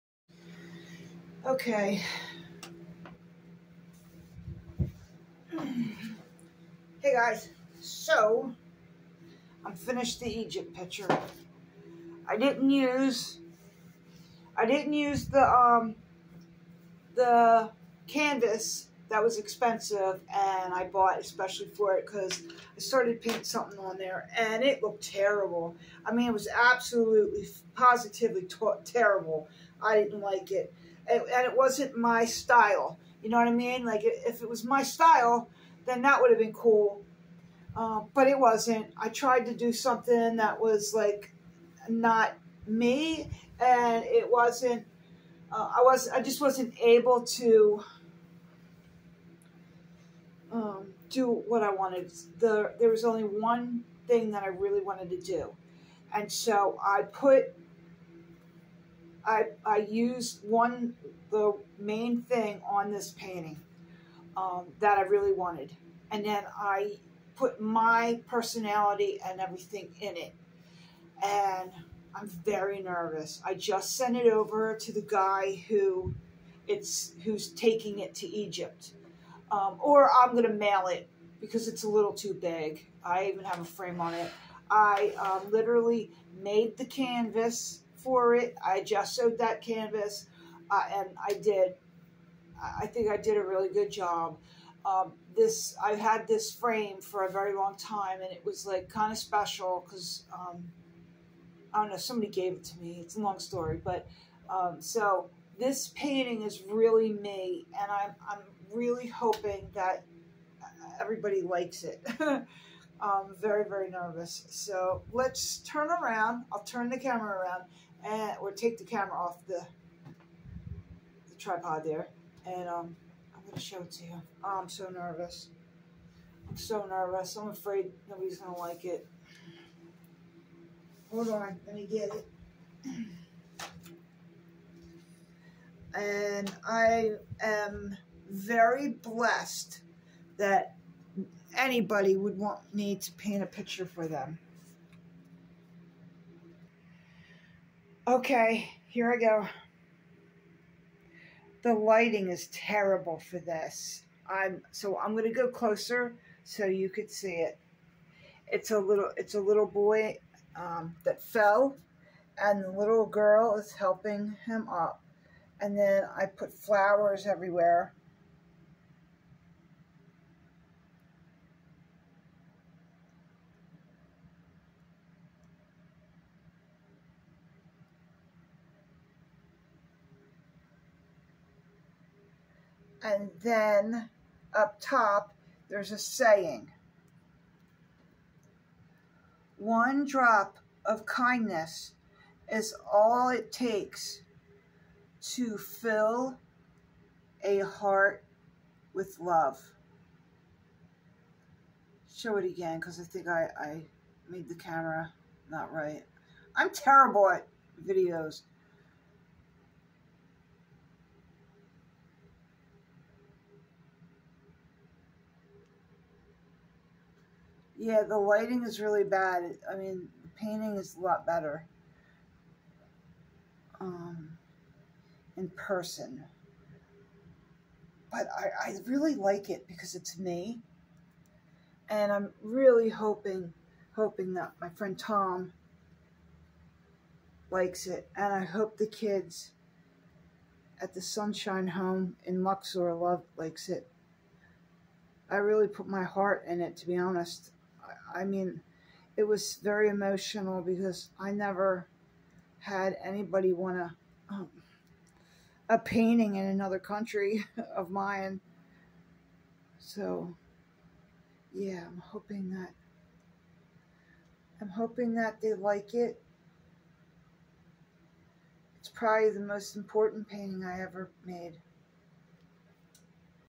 Sunshine Home Of Luxor Egypt Painting is complete ✅
I want to thank my friend Tom for giving me this opportunity to do something very special.. I'm humbled by it. thank you and I hope the children like it too. Maybe they will smile when they see it.